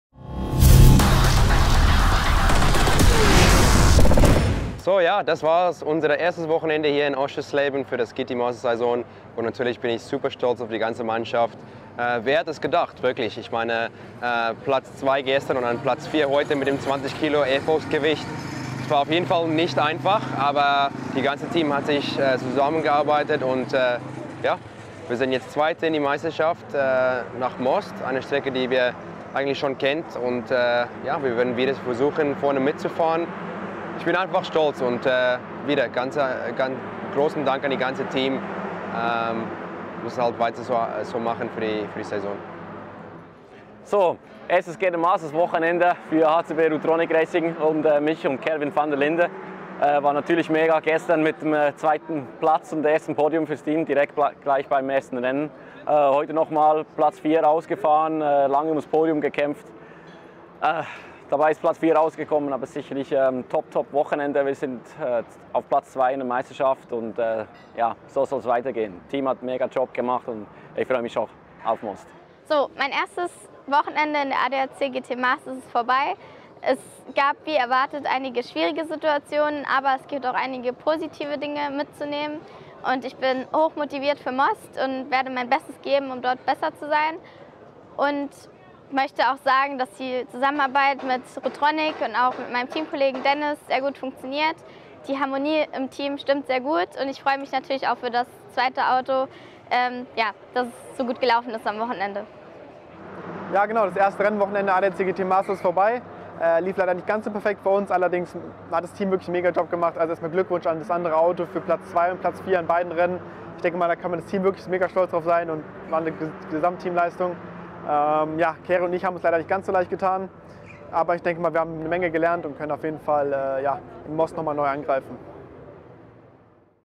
Der HCB-Rutronik Racing Fahrerkader im Interview
Kelvin vd Linde, Patric Niederhauser, Carrie Schreiner und Dennis Marschall - nach Ihrem ersten Wochenende im ADAC GT Masters